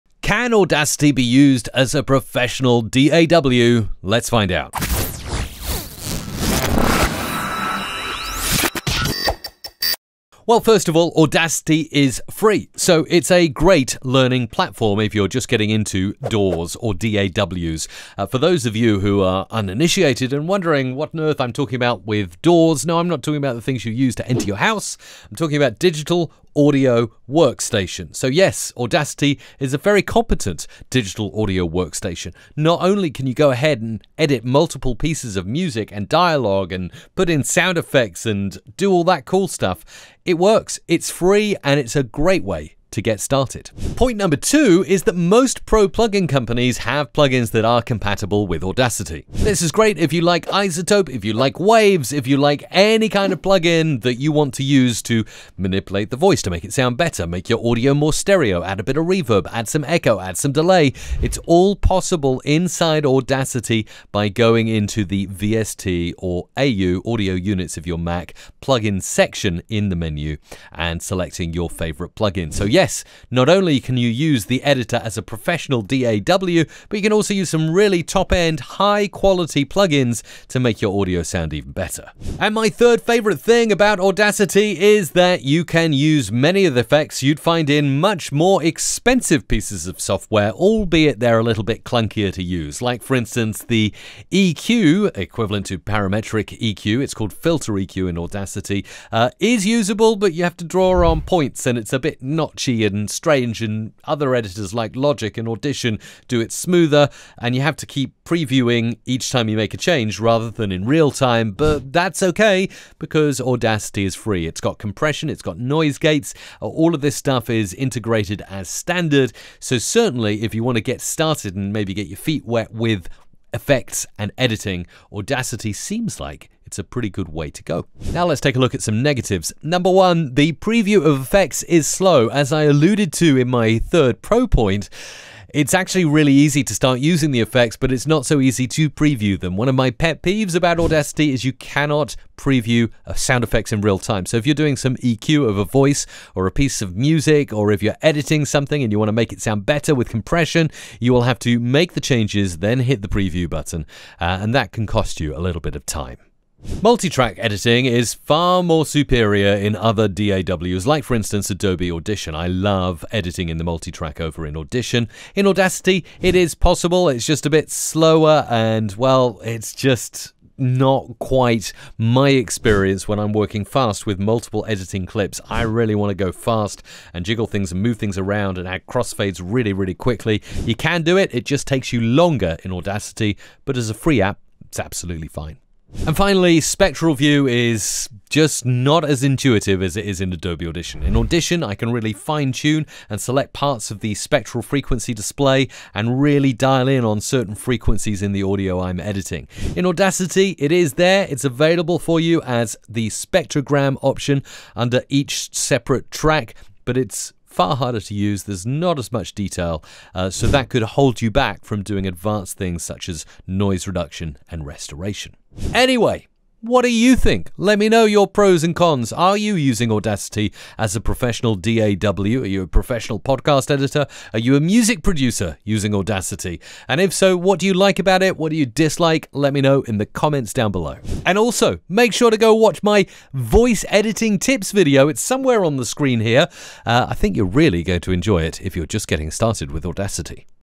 Audacity as a Great Professional DAW? | Audacity PROS and CONS - Producer's HONEST Take on Audacity
Are you still having doubts if Audacity is a good DAW for you?
Let's answer that question in this video.

In this new episode, we will talk about how good Audacity is at editing sounds. I'm sure most of you are searching about these questions - Is it compatible with certain plug-ins? Does it have free audio effects? How fast it is compared to other audio software? Is it worth it? All your doubts will be cleared once you finish watching this video.

◀ 🎤 Audacity Presets! :headphones: The audio presets I use.
◀ 🎚️ Learn how to master audio production using Audacity Audition.

mrc.fm/audacityforbeginners

0:00 Can Audacity be used as a professional DAW?
0:09 Is it DAW Competent?
0:43 Plug-Ins compatibility
1:23 Does it have Free Effects?
2:07 Can it Preview sound effects?
2:35 How fast it is?
3:05 Spectral view
3:35 Final Thoughts?
3:50 Watch my Voice Editing Tips video!